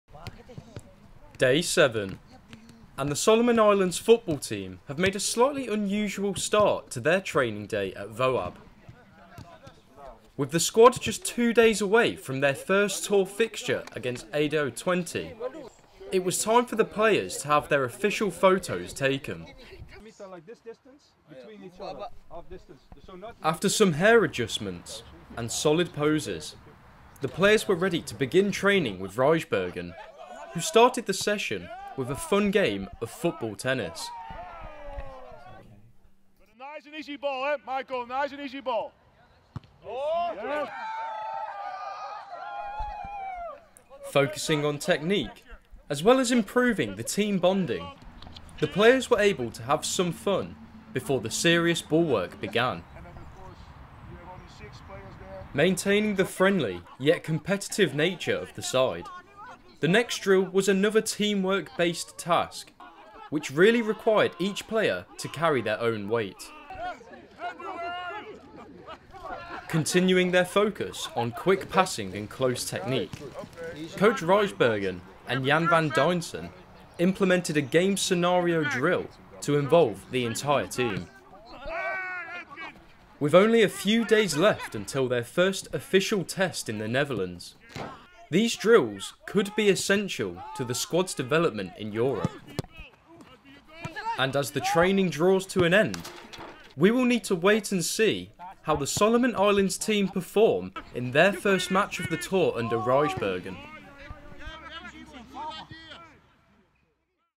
The Solomon Islands National Football Team | Training Camp Day 7. Smile for the Camera
Monday morning saw the boys get ready for the official squad photo and individual profile pictures before Wim Rijsbergen continued to increase the intensity of the training camp.